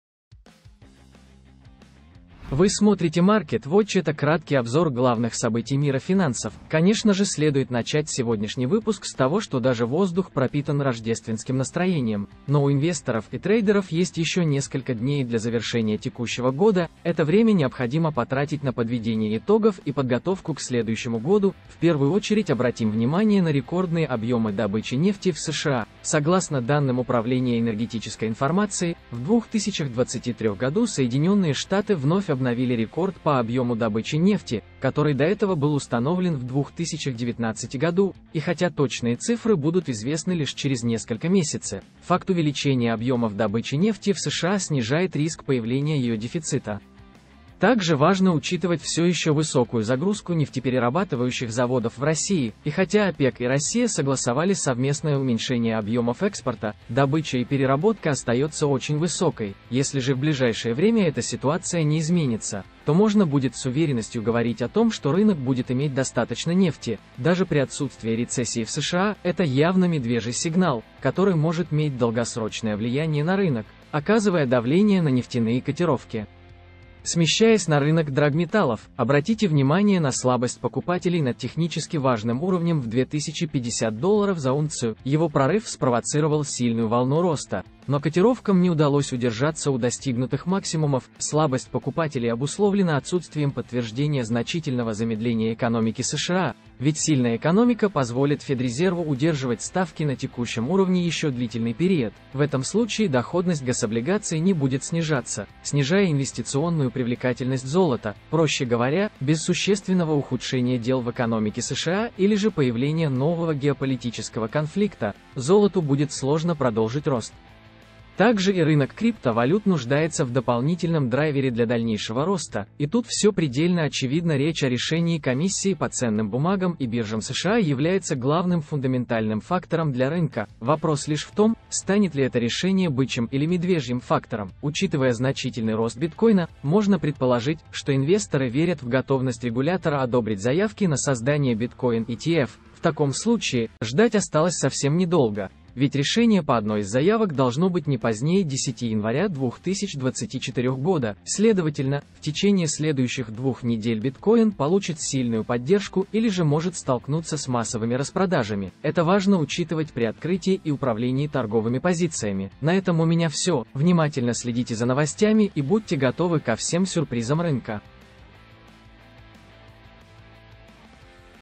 Market Watch. Дефицита нефти не будет
Стань трейдером вместе с ФИБО Групп! Начни торговлю без риска

🔴 В сегодняшнем выпуске рассмотрим следующие темы:

00:00 | Market Watch. Дефицита нефти не будет
00:07 | Дефицита нефти не будет
00:50 | Россия увеличивает объём переработки нефти
01:17 | Золото – спрос остаётся слабым
02:00 | Биткоин в ожидании подарка от SEC

Конечно же следует начать сегодняшний выпуск с того, что даже воздух пропитан рождественским настроением, но у инвесторов и трейдеров есть ещё несколько дней для завершения текущего года. Это время необходимо потратить на подведение итогов и подготовку к следующему году. В первую очередь обратим внимание на рекордные объёмы добычи нефти в США. Согласно данным Управления энергетической информации, в 2023 году Соединенные Штаты вновь обновили рекорд по объёму добычи нефти, который до этого был обновлен в 2019 году. И хотя точные цифры будут известны лишь через несколько месяце, факт увеличения объёмов добычи нефти в США снижает риск появления её дефицита.

Россия увеличивает объём переработки нефти

Также важно учитывать всё ещё высокую загрузку нефтеперерабатывающих заводов в России. И хотя ОПЕК и Россия согласовали совместное уменьшение объёмов экспорта, добыча и переработка остаётся очень высокой. Если же в ближайшее время эта ситуация не изменится, то можно будет с уверенностью говорить о том, что рынок будет иметь достаточно нефти, даже при отсутствии рецессии в США. Это явно медвежий сигнал, который может меть долгосрочное влияние на рынок, оказывая давление на нефтяные котировки.

Золото – спрос остаётся слабым

Смещаясь на рынок драгметаллов, обратите внимание на слабость покупателей над технически важным уровнем в 2050 долларов за унцию. Его прорыв спровоцировал сильную волну роста, но котировкам не удалось удержаться у достигнутых максимумов. Слабость покупателей обусловлена отсутствием подтверждения значительного замедления экономики США. Ведь сильная экономика позволит Федрезерву удерживать ставки на текущем уровне ещё длительный период. В этом случае доходность гособлигаций не будет снижаться, уменьшая инвестиционную привлекательность золота. Проще говоря, без существенного ухудшения дел в экономике США или же появления нового геополитического конфликта, золоту будет сложно продолжить рост.

Биткоин в ожидании подарка от SEC

Также и рынок криптовалют нуждается в дополнительном драйвере для дальнейшего роста. И тут всё предельно очевидно – решение Комиссии по ценным бумагам и биржам США является главным фундаментальным фактором для рынка. Вопрос лишь в том, станет ли это решение бычьим или медвежьим фактором? Учитывая значительный рост биткоина, можно предположить, что инвесторы верят в готовность регулятора одобрить заявки на создание биткоин-ETF. В таком случае, ждать осталось совсем не долго, ведь решение по одной из заявок должно быть оглашено не позднее 10 января 2024 года. Следовательно, в течение следующих двух недель биткоин получит сильную поддержку или же может столкнуться с массовыми распродажами. Это важно учитывать при открытии и управлении торговыми позициями.

Просто, понятно и доступно о финансовом анализе и свежие обзоры рынка в